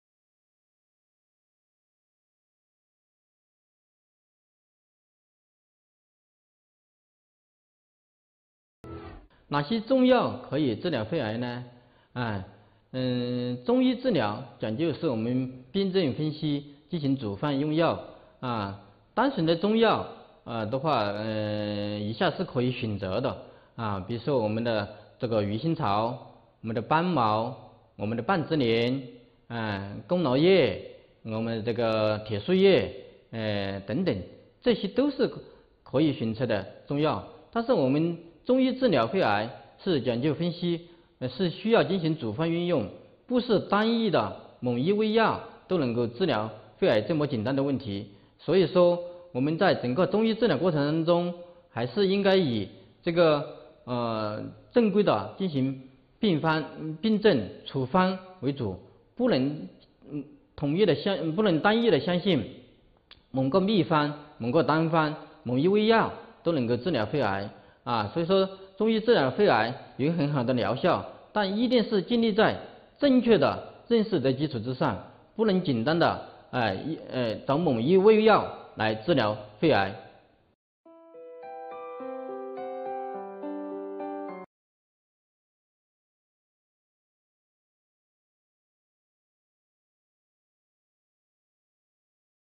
治疗肺癌的中药有哪些 杨常清 重庆市綦江区人民医院
治疗肺癌的中药有：中医治疗讲究的是病症分析进行组方用药。单纯的中药有鱼腥草、斑蝥、半枝莲、功劳叶、铁树叶等这些都是可以选择，但是中医治疗肺癌是需要进行组方用药，不是单一的某一味药都能够治疗肺癌，所以中医治疗过程中，应该正规的进行病症处方为主。

What are the traditional Chinese medicine for lung cancer
The traditional Chinese medicine for lung cancer is as follows: the treatment of traditional Chinese medicine pays attention to the analysis of disease and the formation of prescriptions. Pure Chinese medicine includes Houttuynia cordata, cantharides, Scutellaria barbata, Mahonia leaves, iron leaves, etc, But TCM treatment of lung cancer requires prescription, Not a single drug can treat lung cancer, So in the process of TCM treatment, Should carry on the disease prescription to give priority to formally.